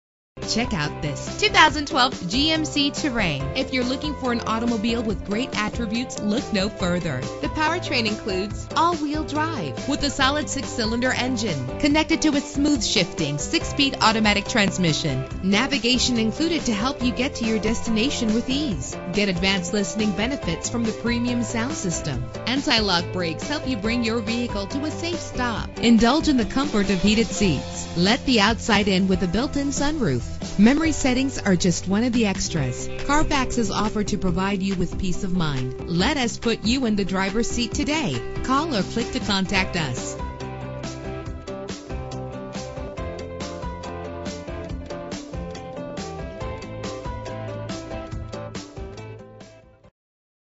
2012 GMC Terrain - Lafayette IN
Year: 2012
Make: GMC
Model: Terrain
Trim: SLT-2
Engine: 3.0 liter 6 cylinder 24 valve GDI DOHC
Transmission: 6-Speed Automatic
Color: Quicksilver Metallic
Mileage: 20149
Address:
2051 Sagamore Pkwy. S.